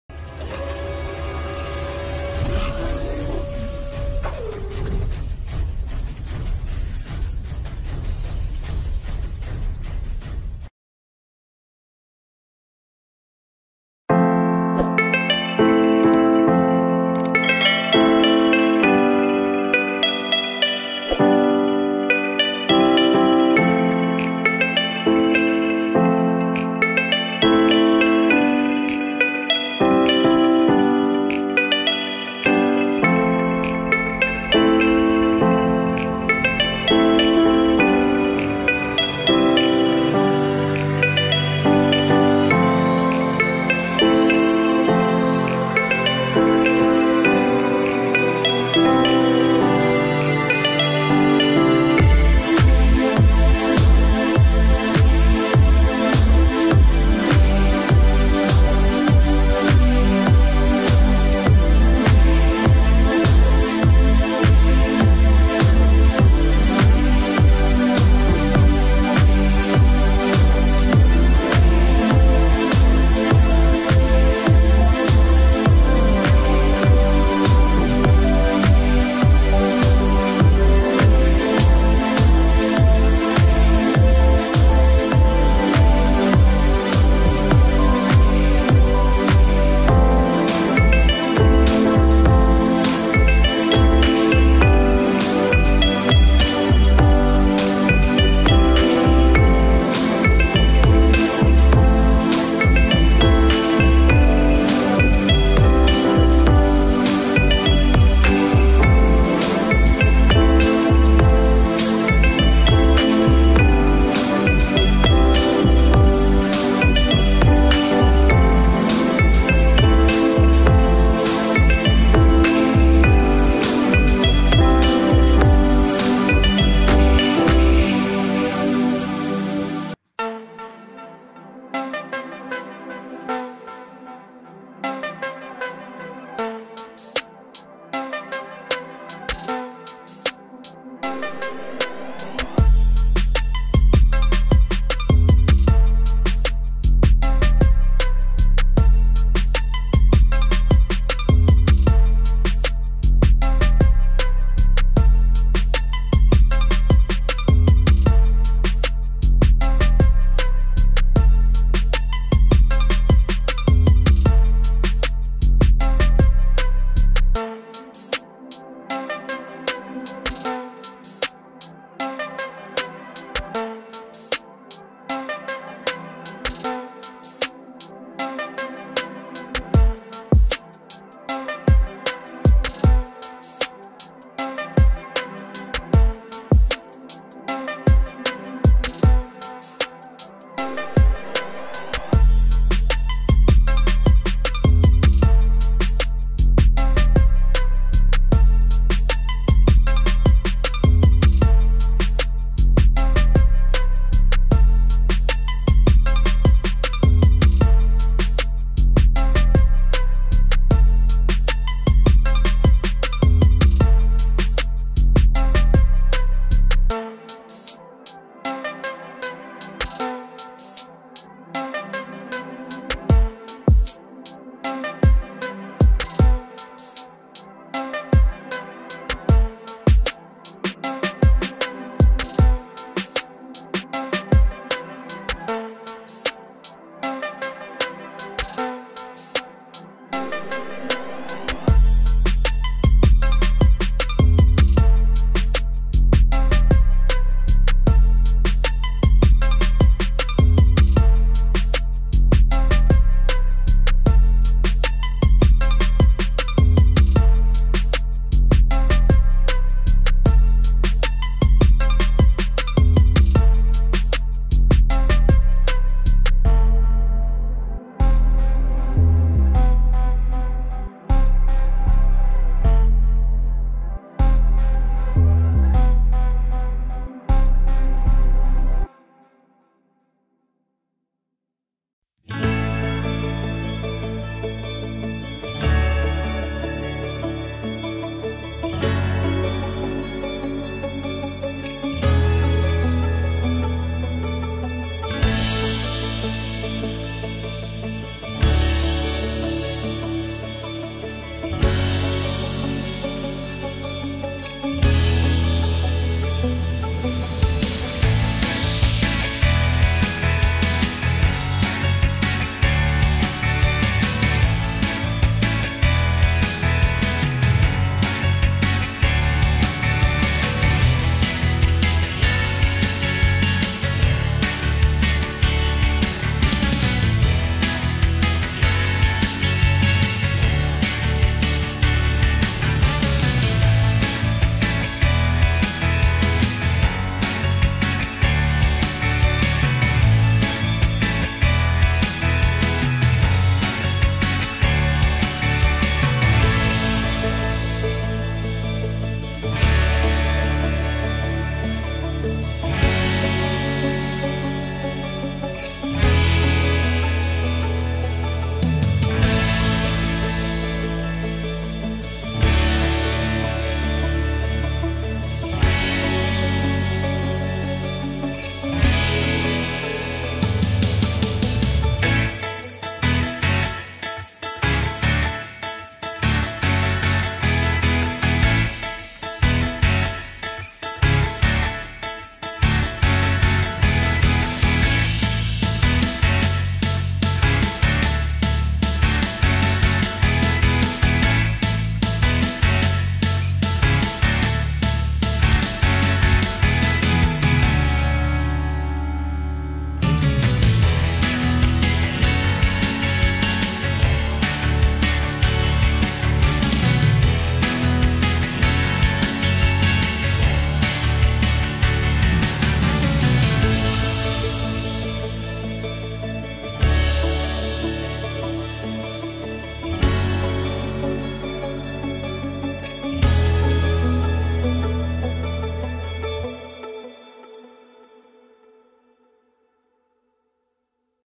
Resize RootFS partition openwrt
Aktifkan  fitur CC/Substitle di video ini untuk keterangan detail.

Merubah nilai partisi flashdisk yang sudah berisi fw openwrt dengan Armbian linux ubuntu.
Software yang dipergunakan Gparted linux
Berguna jika ingin merubah nilai partisi atau membuat partisi baru .